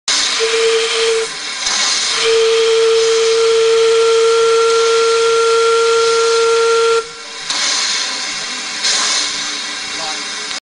Steam Engine's Whistle
Helen Kathryn's Whistle blowing.
In South Tynedale Railway.